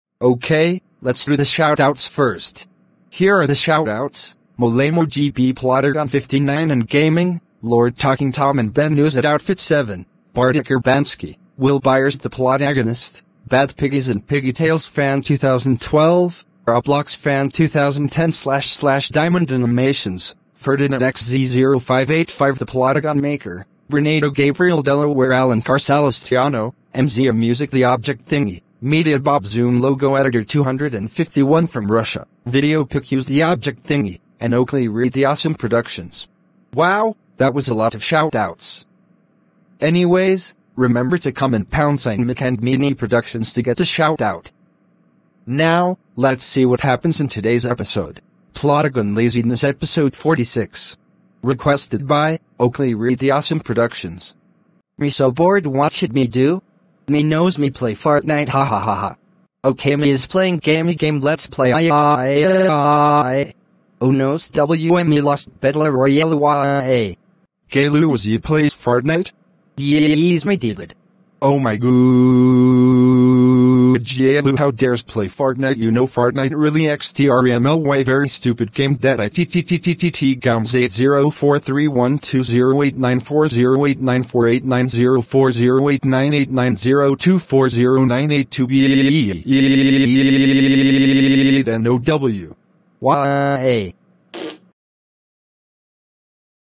Plotagon Laziness Episode 46: "Gayloo Play Fartnite"
Gayloo plays Fortnite, nothing special